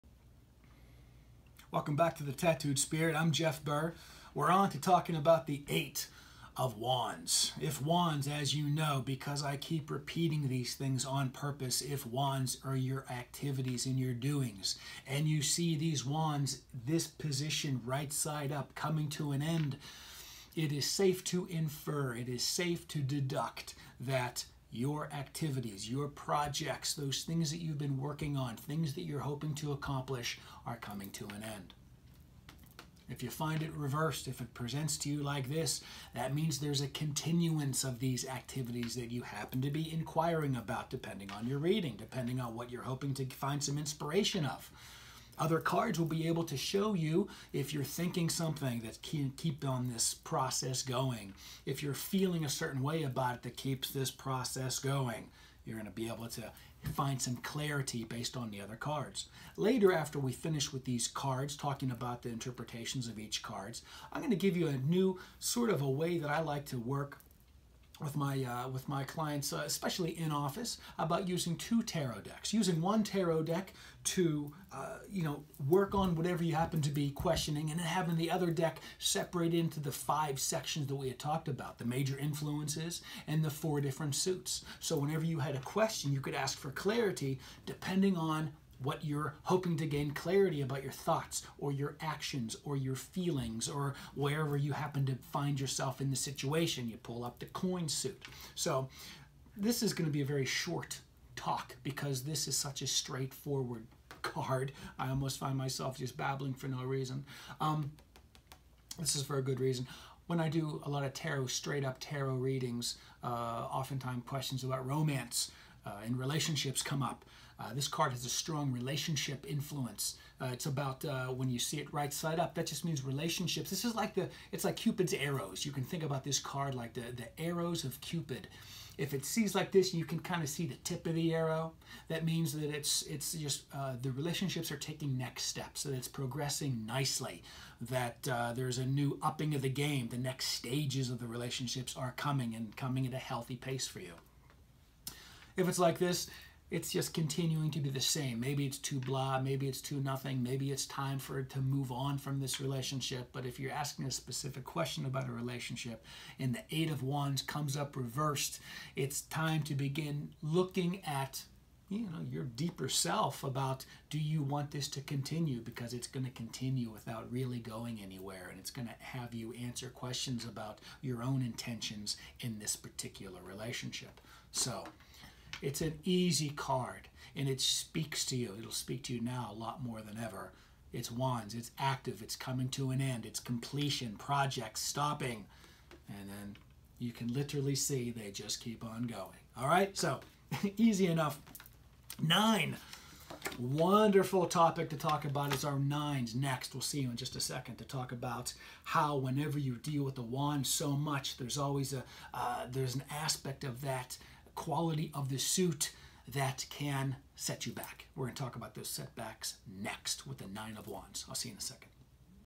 [30] EIGHT of WANDS
ATTENTION: The Tarot MASTERCLASS is now available. Journey with me from these basic tarot meanings to expert tarot readings!

View the ease in resolving the meaning of this wonderful 8.  All 8's have something to do with movement (or the lack of movement), and seeing that these actions (Wands) are coming to an end...well, there's your answer on the subject in which you inquired.  Could the reversed meaning of this card be as simple as that...only in reverse?  Hit play and find out!